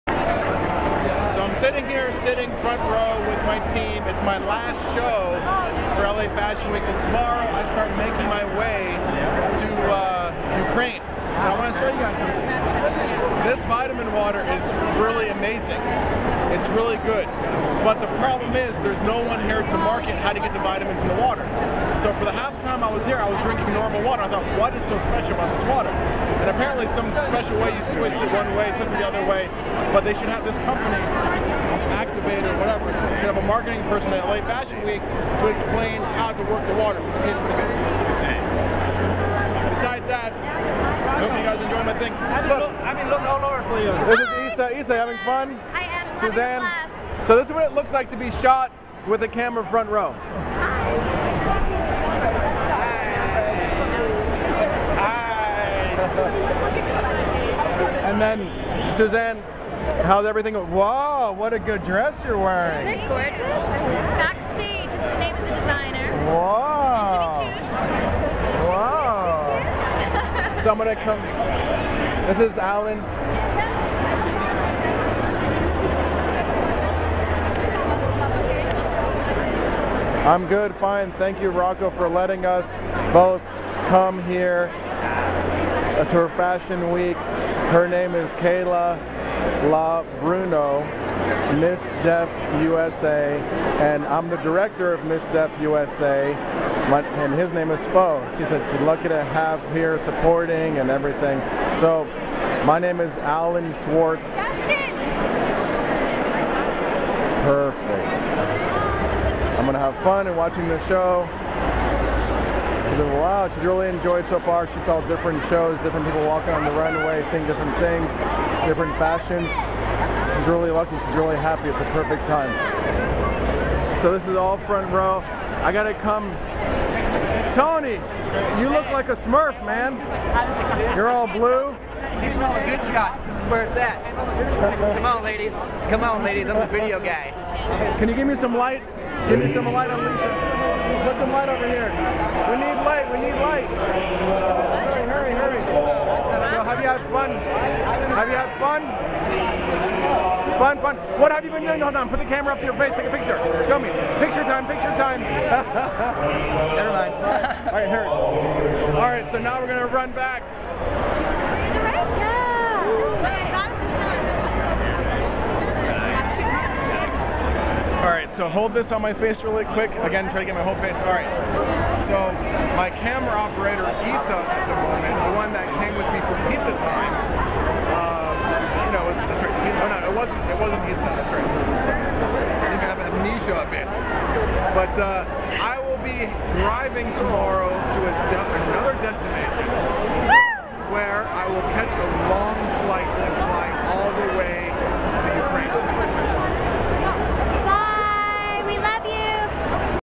Rocco's Vlog: Rocco Tackles a Water Bottle and More
Rocco has fun on his last night of Fashion Week with his FAB crew and his VERY special guests Alan Schwartz, the Director of the Miss Deaf USA Pageant and Miss Deaf USA Kayla LaBruno before he heads off to the UKRAINE!